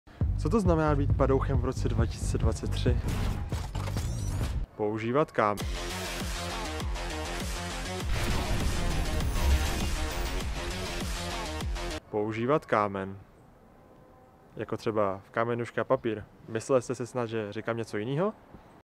PADOUCH VS PADOUCH - TEASER | miniFILM 2023
Čau lidi, po více jak roce jsem zpět a přináším vám teaser na připravovaný minifilm na kterém se postupně pracuje už přes 2 roky. Jak jde z názvu videa poznat, minifilm by měl vyjít další rok.
Moc doufám, že se vám bude líbit a před jeho vydáním (a i po) mám připravená další videa, takže je na co se těšit!

Tento teaser byl mimochodem parodie na memy, které vznikaly když vydal CD Projekt Red teaser na hru Cyberpunk 2077. Originál je zde

Programy ve kterých edituji:
- Photoshop
- After Effects
- Premiere Pro

Děkuju za zhlédnutí videa :)